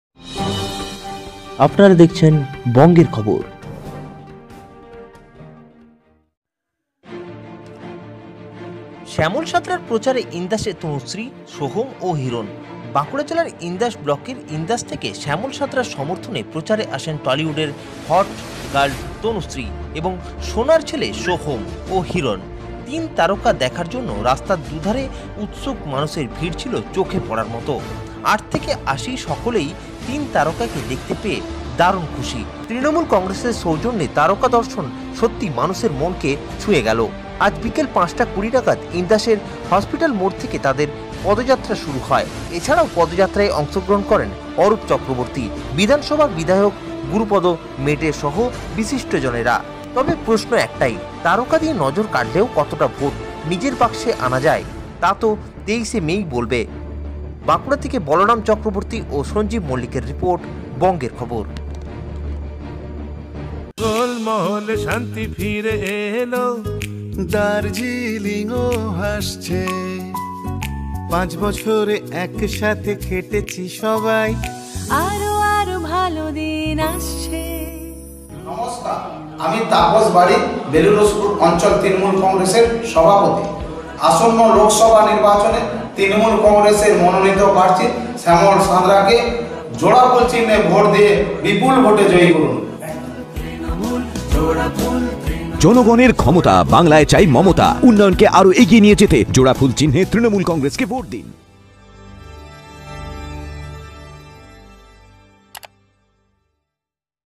বাঁকুড়া : শ্যামল সাঁতরার প্রচারে ইন্দাসে তনুশ্রী , সোহম ও হিরণ । bonger khobor
বাঁকুড়া : শ্যামল সাঁতরার প্রচারে  ইন্দাসে তনুশ্রী , সোহম ও হিরণ ।

সঞ্জীব মল্লিক ও বলরাম চক্রবর্ত্তী  , বাঁকুড়া  :